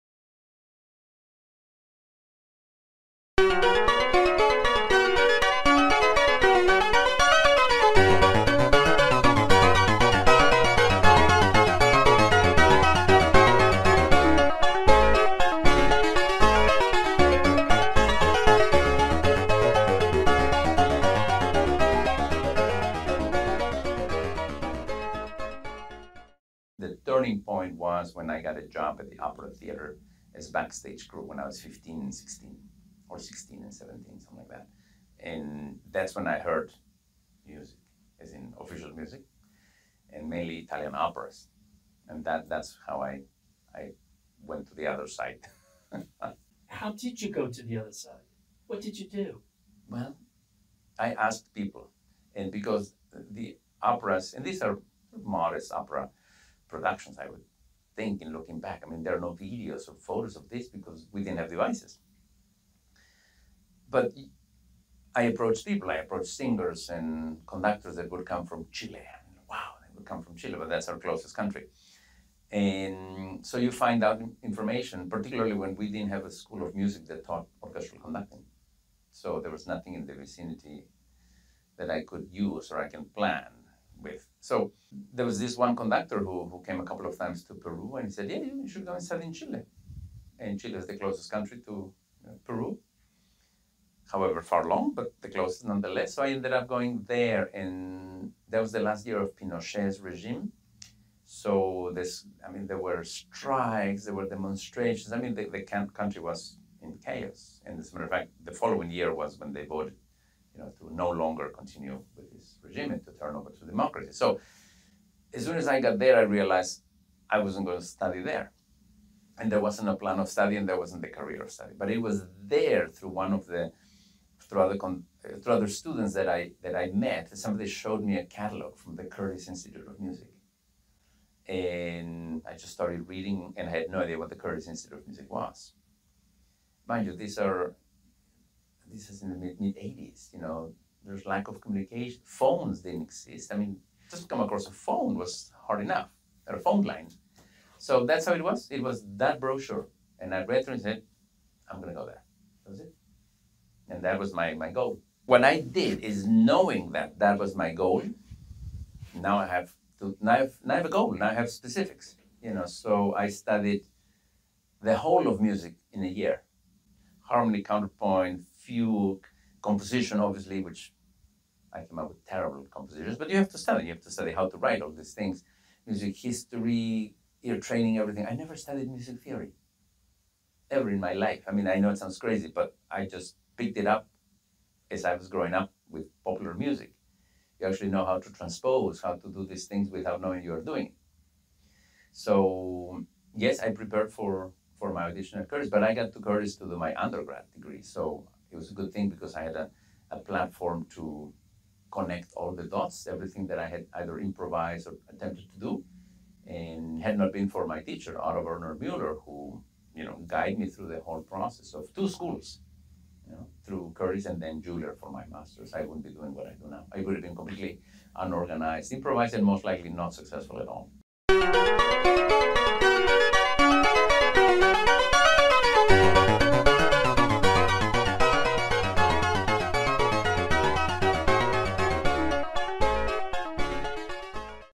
The short of it: How Miguel Harth-Bedoya Set His Course in Music
This is a focused look at how Maestro Harth-Bedoya started his career in conducting.